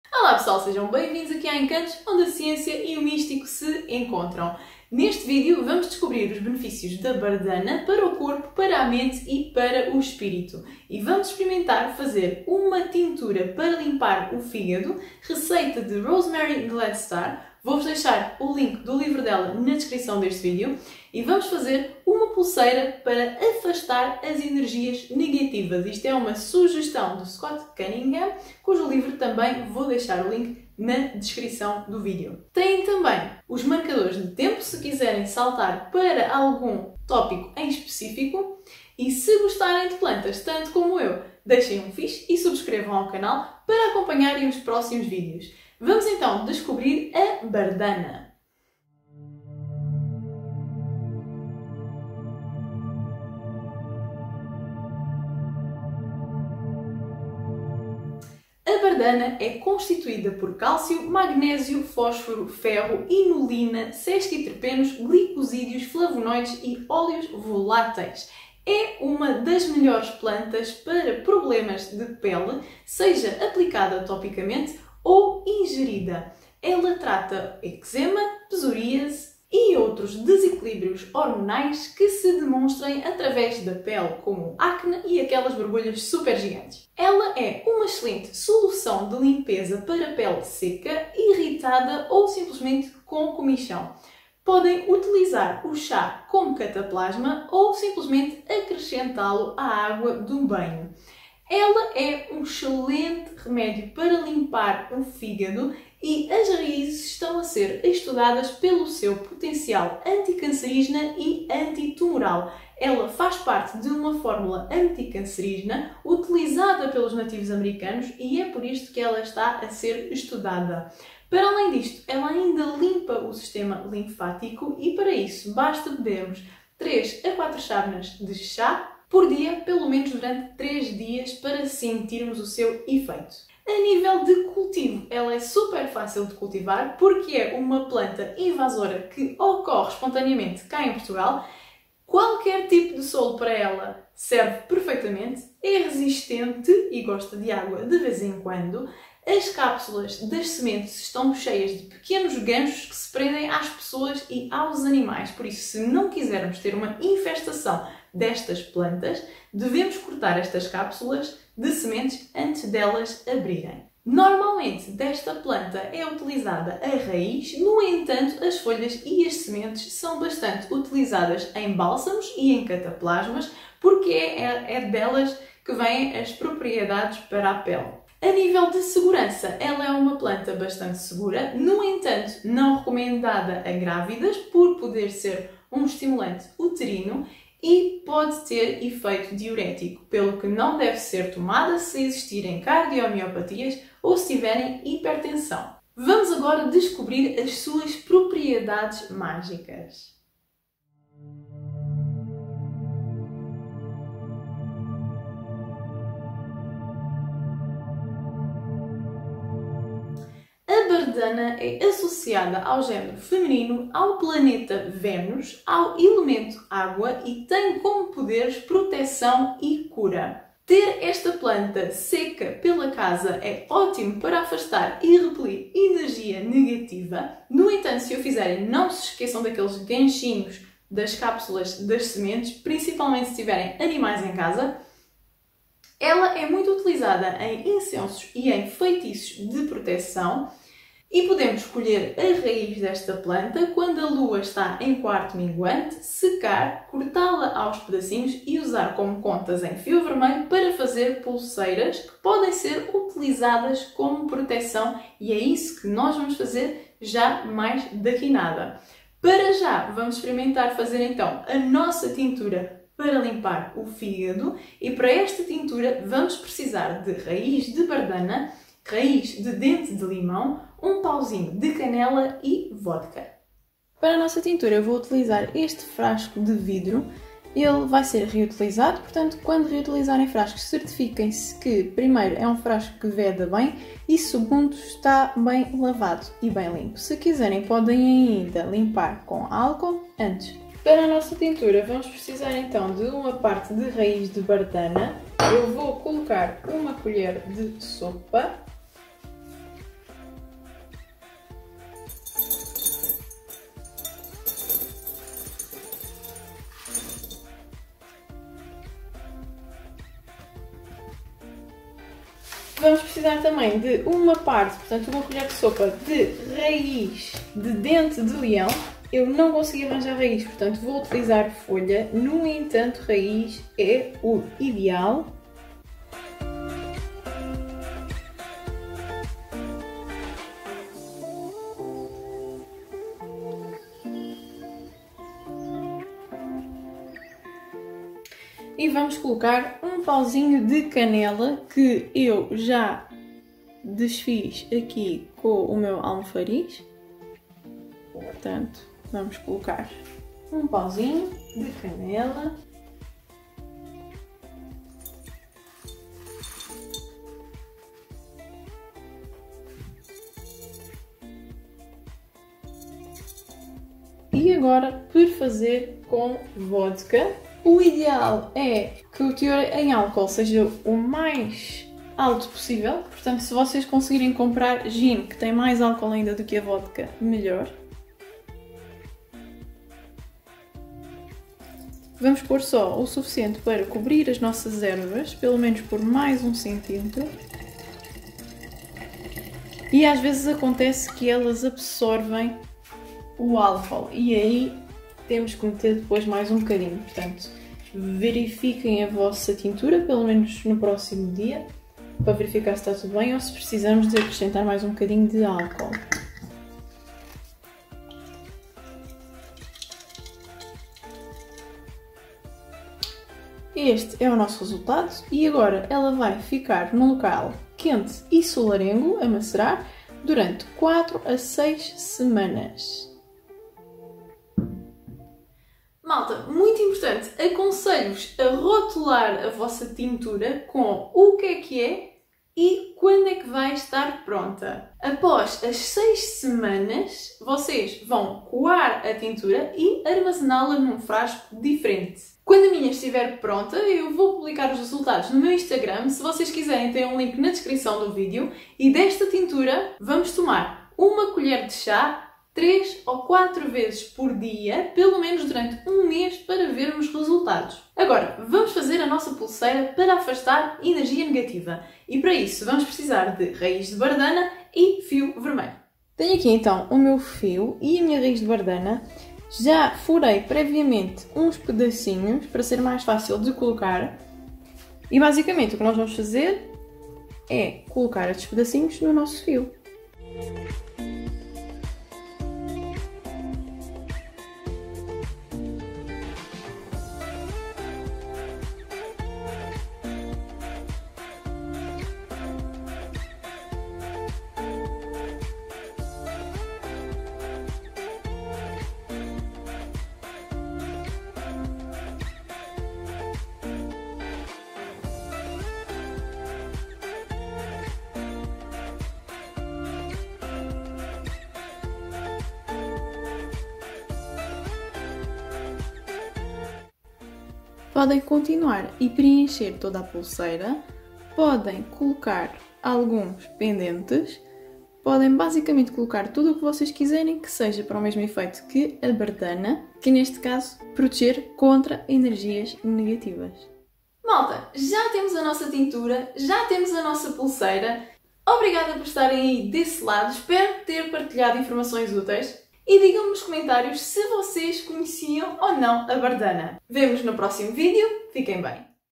BARDANA para a Pele e o Fígado
A Bardana é muito boa para a Pele e para o Fígado.
Descobre para que serve a raiz e a folha da Bardana, se tem efeitos colaterais e como podes utilizar.
A Bardana é muito boa para qualquer irritação a nível da pele, eczema, psoríase, comichão e acne por exemplo.
Ela também é excelente para limpar o sistema linfático e trabalha muito bem em conjunto com o dente de leão para limpeza do fígado.
Para além disto ela ainda está a ser estudada pelas suas propriedades anticancerígenas e anti tumorais.

Na magia ela é considerada uma planta de proteção e cura, muito utilizada em incensos e feitiços de proteção e colocada pela casa para proteção do lar.

Neste vídeo vamos fazer uma tintura para limpeza do sistema linfático e uma pulseira de proteção contra energias negativas.

Obrigada :)

Tempos

00:00 Intro
00:45 Propriedades Terapêuticas
03:16 Propriedades Mágicas
04:20 Tintura Limpeza Fígado
07:37 Pulseira Proteção Energias Negativas
08:54 Outro

A receita da Tintura é da Rosemary Gladstar, no livro "Medicinal Herbs".
A ideia da pulseira é de Scott Cuningham, no livro "Encyclopedia of Magical Herbs":